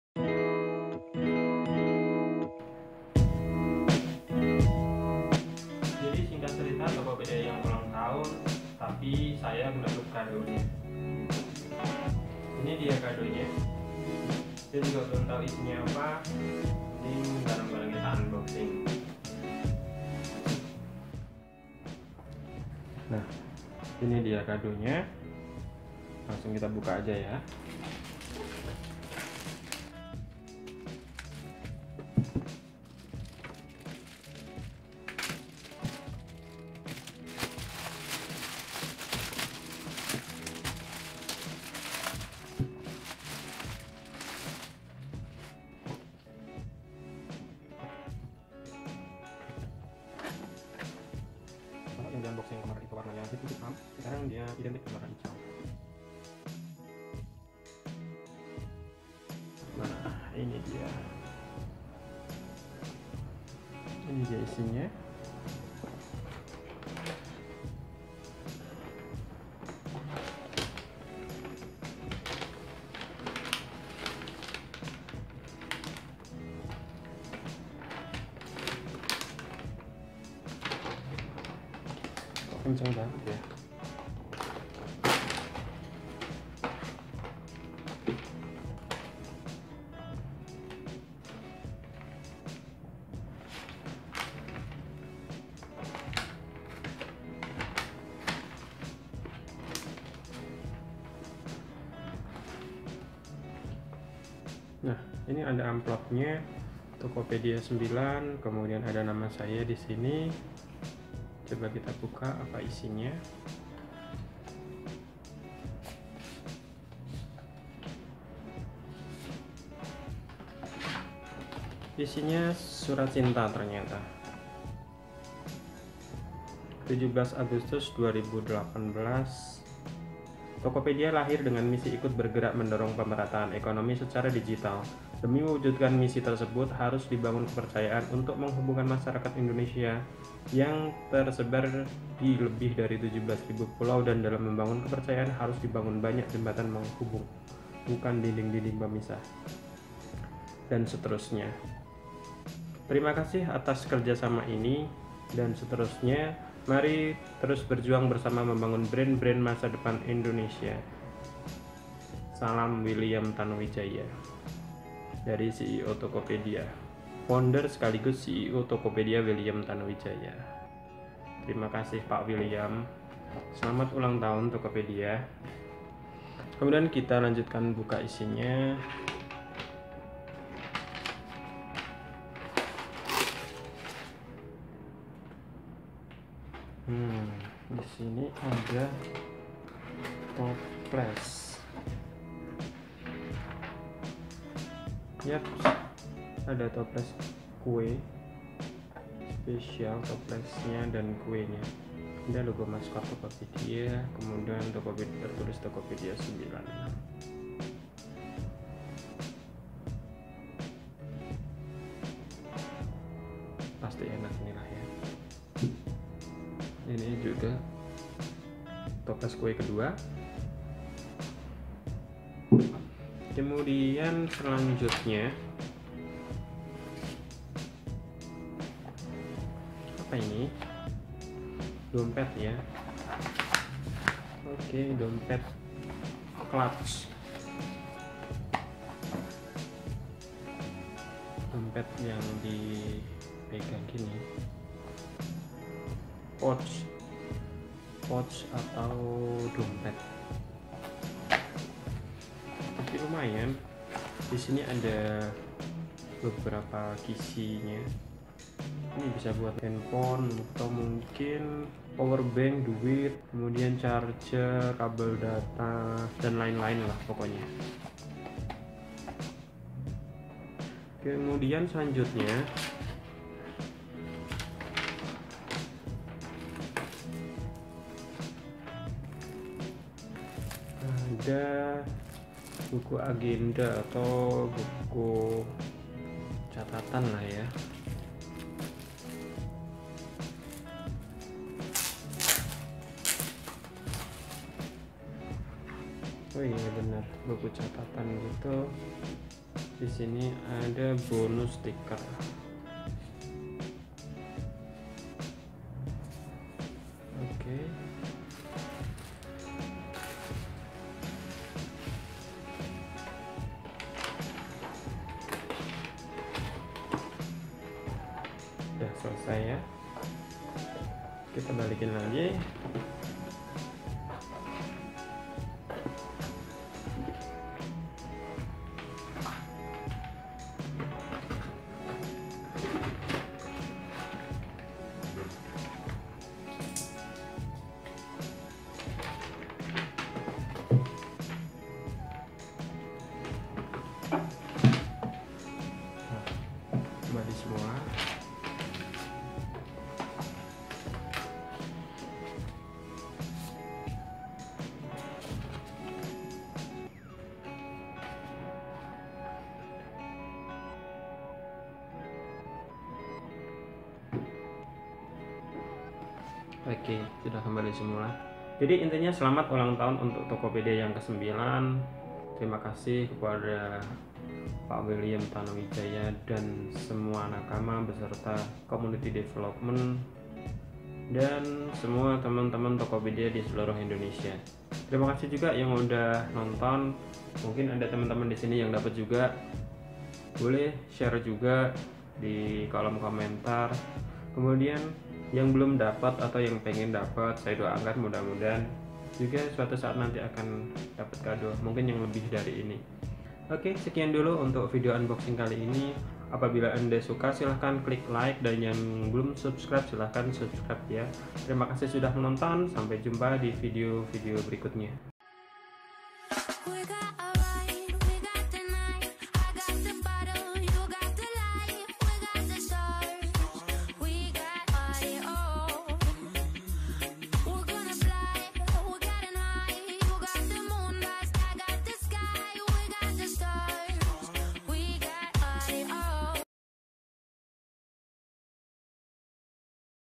Unboxing Kado ULANG TAHUN Anniversary 9 TOKOPEDIA
Maaf, very late post, sampai ganti tahun.. harap maklum..